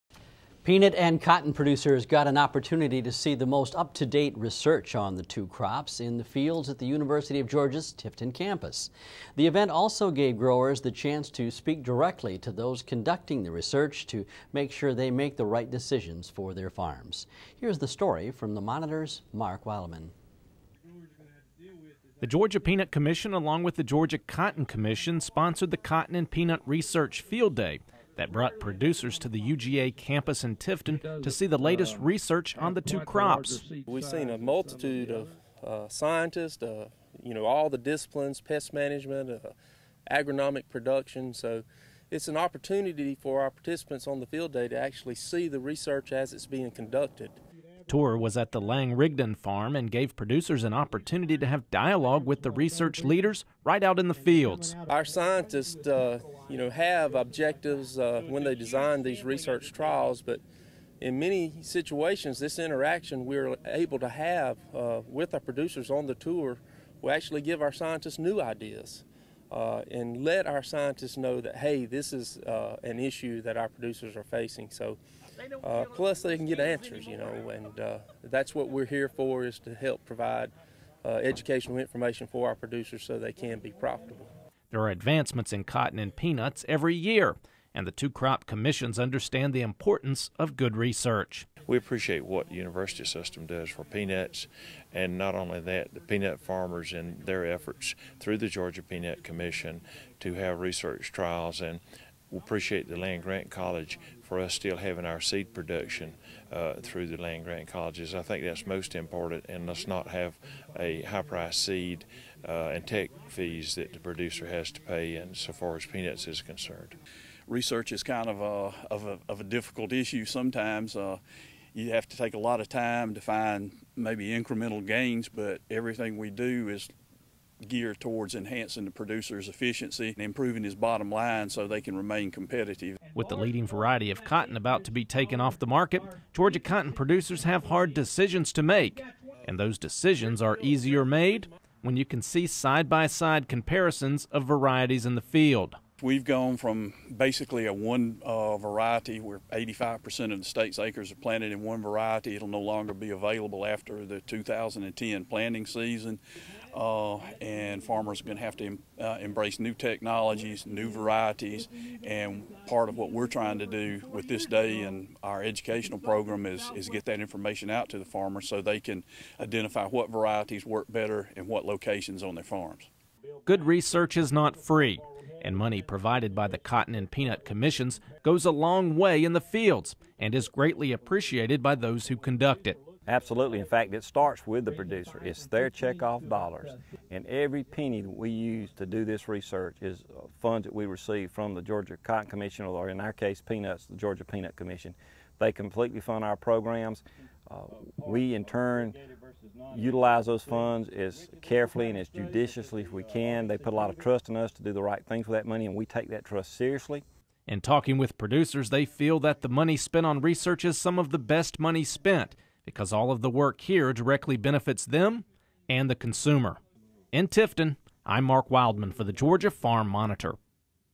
Peanut & Cotton Field Day
Peanut and cotton producers got an opportunity to see the most up to date research on the two crops in the fields at the University of Georgia's Tifton Campus.  It also gave growers the chance to speak directly to those conducting the research to ensure growers make the right decisions for their farms.  Here's the story from the Monitor's Mark Wildman.